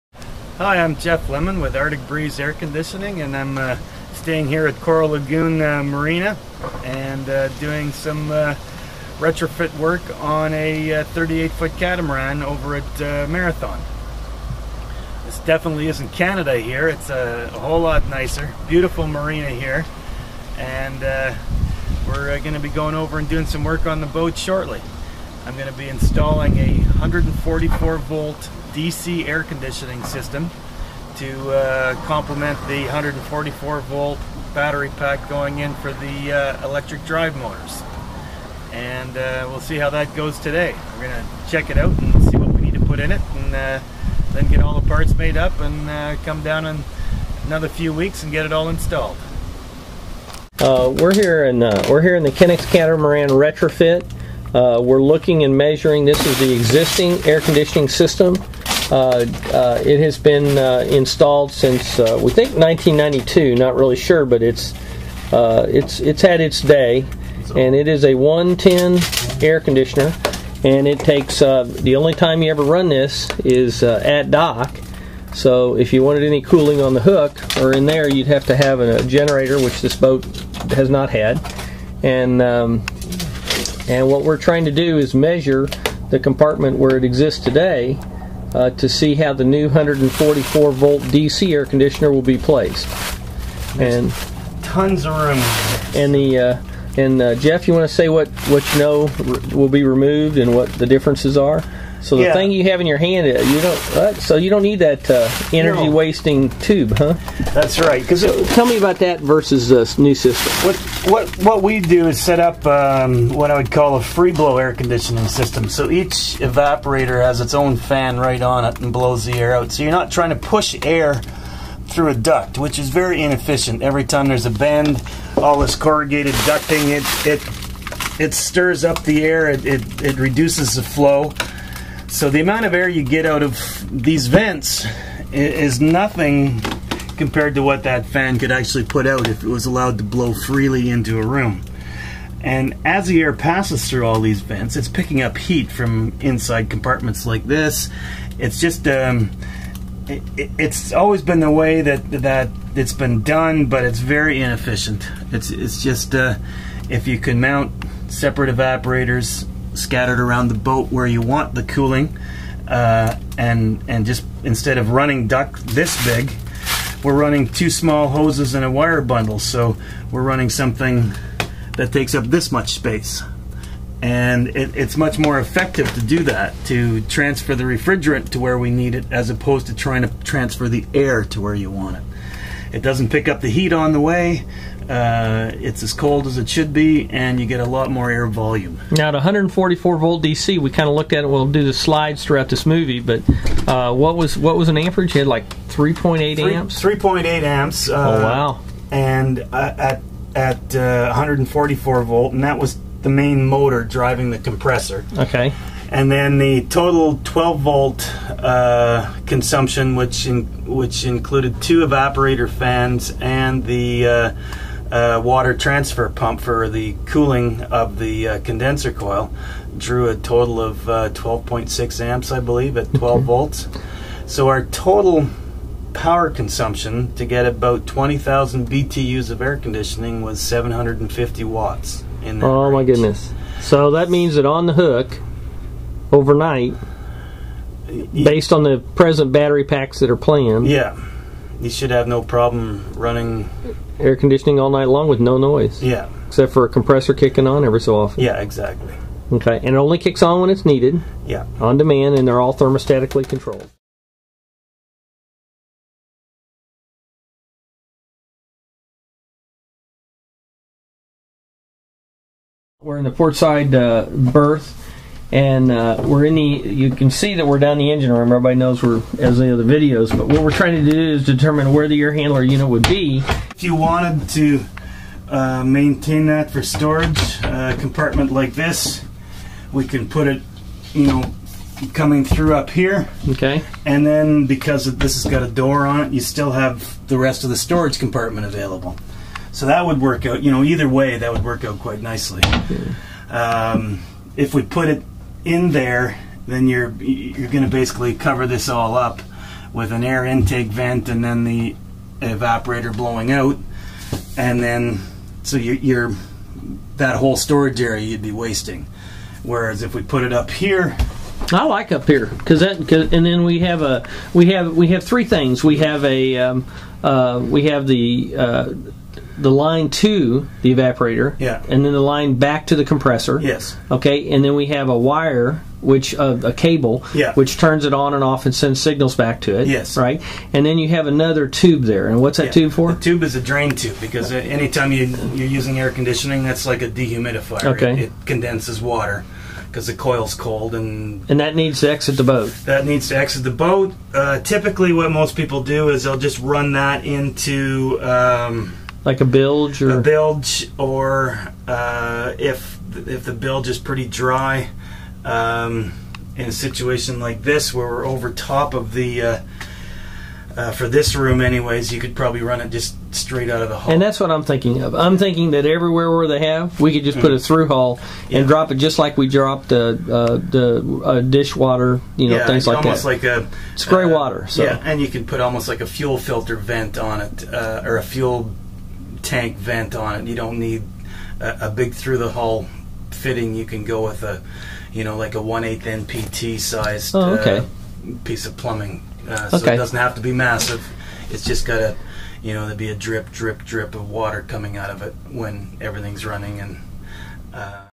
Hammond Artic Wave 144V dc Airconditioning Solutions4Hybrids.com
THis is the beginning of the 144V dc revolution in marine propulsion and creature comforts.  This is the manufacturer of the air-conditioning system using Efficiencies  with simplicity to make the cooling potential of the America Sailing Clubs Genigreen Eelectric Catamaran have a drop in temperature of 30 degrees...WOW!  Solutions4Hybrids.com